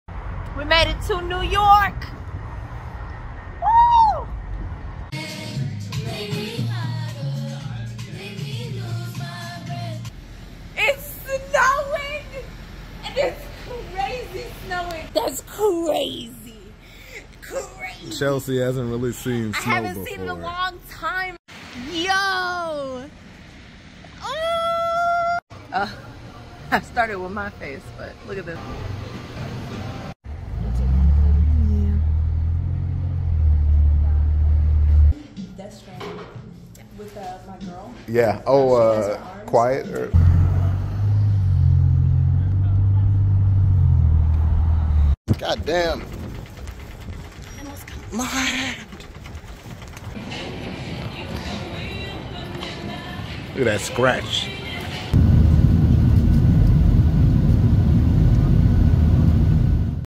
1 Minute Vlog - We're in New York and IT'S SNOWING!
Join us for a 1-minute vlog as we explore New York City in the snow! From trying delicious winter foods to seeing iconic landmarks covered in snow, this quick vlog captures the magic of the city in winter. Don't miss out on our snowy adventures in the Big Apple!

We'd love to have you as a part of the Byte Command.